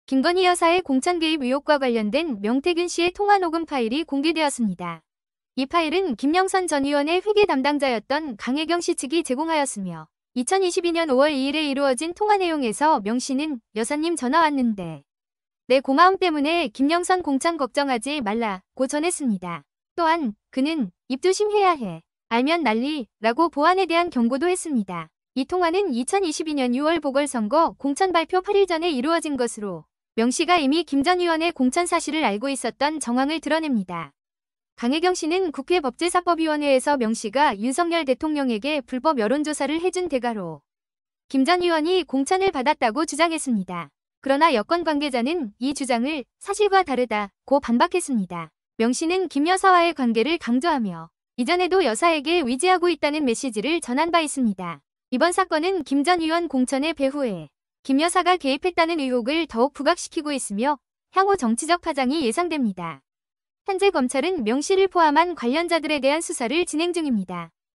'명태균, 김건희 공천은 선물? 통화 녹취 공개로 드러난 의혹'
김건희 여사의 공천 개입 의혹과 관련된 명태균 씨의 통화 녹음 파일이 공개되었습니다. 이 파일은 김영선 전 의원의 회계 담당자였던 강혜경 씨 측이 제공하였으며, 2022년 5월 2일에 이루어진 통화 내용에서 명 씨는 "여사님 전화 왔는데 내 고마움 때문에 김영선 (공천) 걱정하지 말라"고 전했습니다. 또한, 그는 "입조심 해야 해. 알면 난리"라고 보안에 대한 경고도 했습니다.

이 통화는 2022년 6월 보궐선거 공천 발표 8일 전에 이루어진 것으로, 명 씨가 이미 김 전 의원의 공천 사실을 알고 있었던 정황을 드러냅니다. 강혜경 씨는 국회 법제사법위원회에서 명 씨가 윤석열 대통령에게 불법 여론조사를 해 준 대가로 김 전 의원이 공천을 받았다고 주장했습니다. 그러나 여권 관계자는 이 주장을 "사실과 다르다"고 반박했습니다.

명 씨는 김 여사와의 관계를 강조하며, 이전에도 여사에게 의지하고 있다는 메시지를 전한 바 있습니다. 이번 사건은 김 전 의원 공천의 배후에 김 여사가 개입했다는 의혹을 더욱 부각시키고 있으며, 향후 정치적 파장이 예상됩니다. 현재 검찰은 명 씨를 포함한 관련자들에 대한 수사를 진행 중입니다.